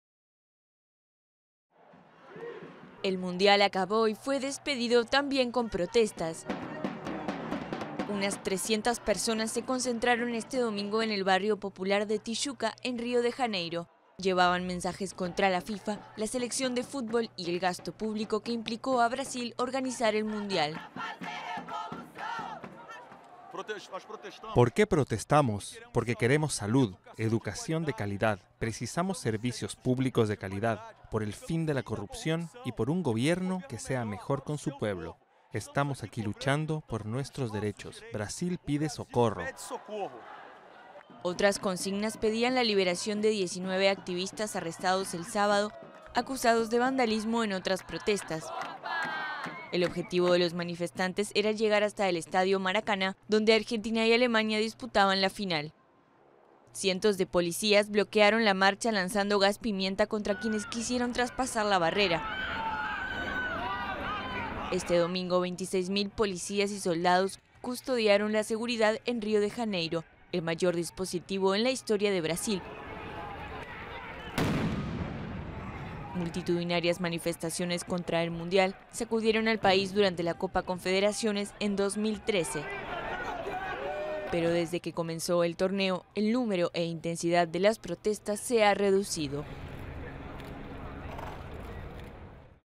Policía dispersa protesta anti Copa
La policía dispersó este domingo con gases lacrimógenos a unos 300 manifestantes antiCopa que querían acercarse al Maracaná minutos antes del inicio de la final del Mundial-2014.